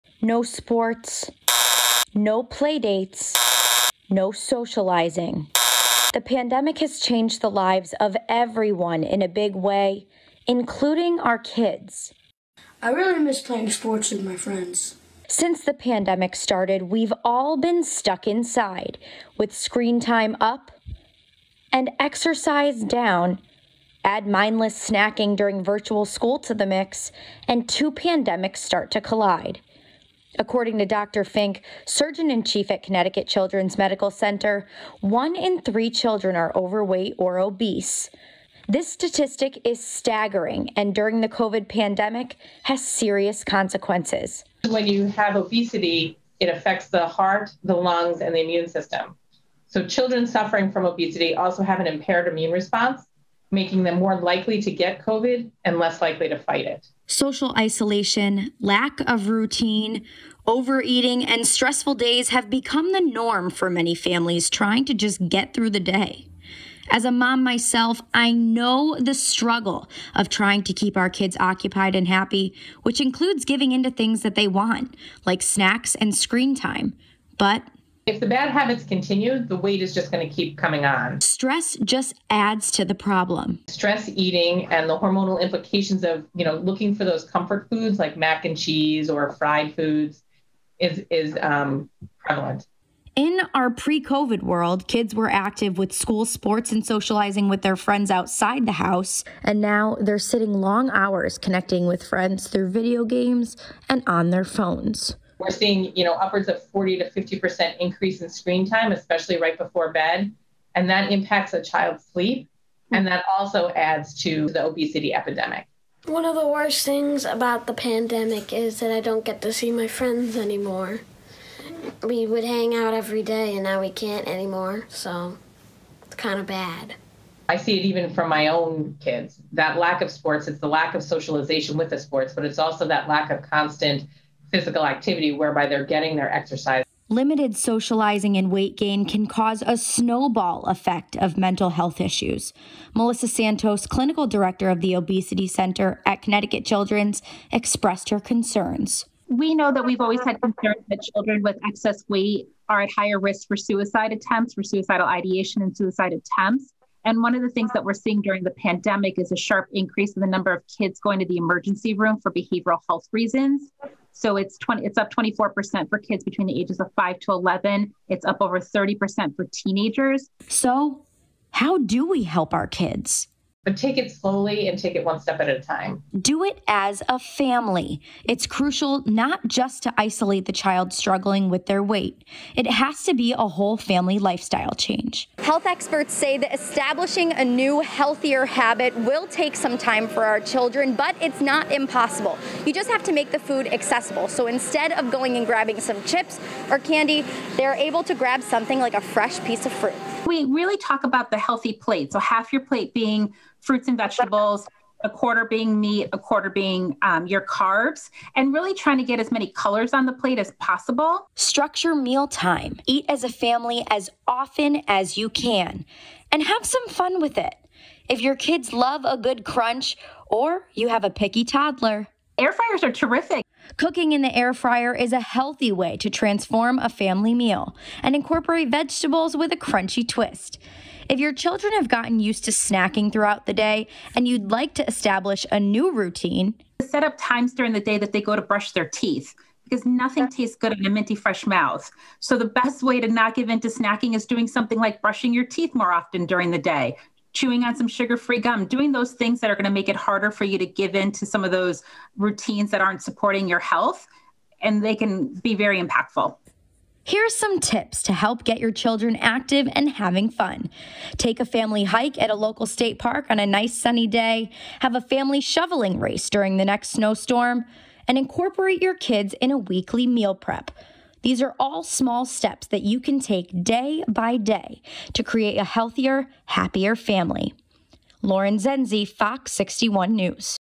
COVID-19 and childhood obesity: When two pandemics collide
According to Neonatal Adiposity and Childhood Obesity, data shows in 2020, 1 in 3 children were considered overweight or obese.